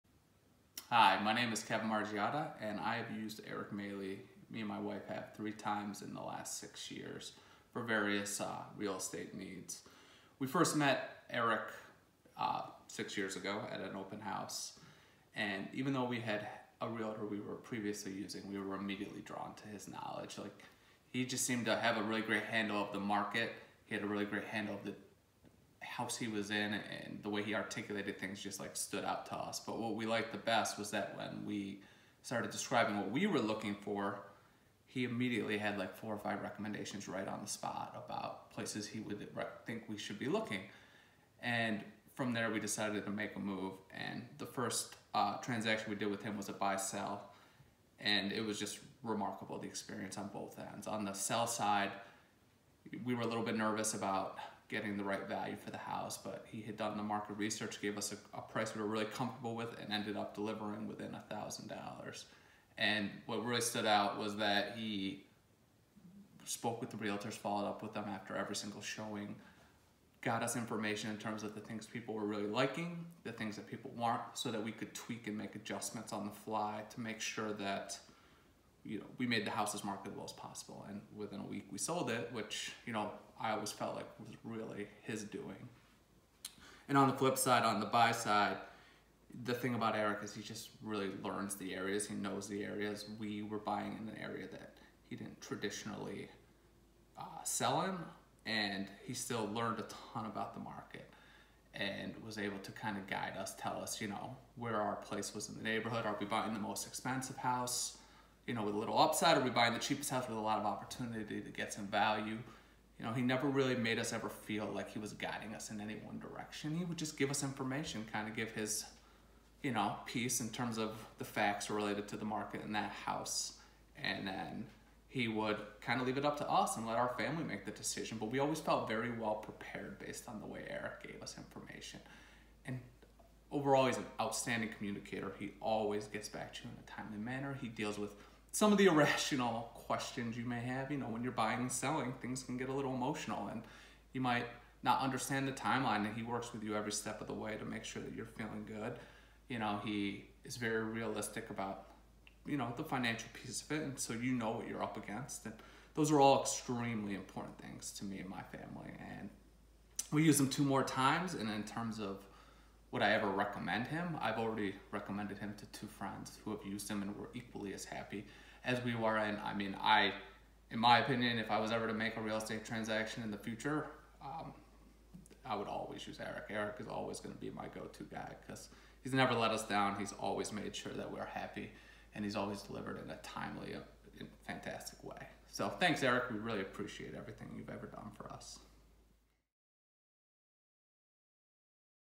Testimonial with Kevin & Becky
Working with Eric was a great experience from start to finish. He was able to sell our home quickly and was extremely helpful in picking out and negotiating our new one. And when some complications occurred in our closing process he put in a lot of extra time and effort to ensure everything kept moving along. Eric is extremely professional, knowledgeable, an excellent communicator, and works as hard as anyone I have come across in the industry. My wife and I are clients for life and would strongly recommend him to any of our family or friends.

Kevin & Becky Margiotta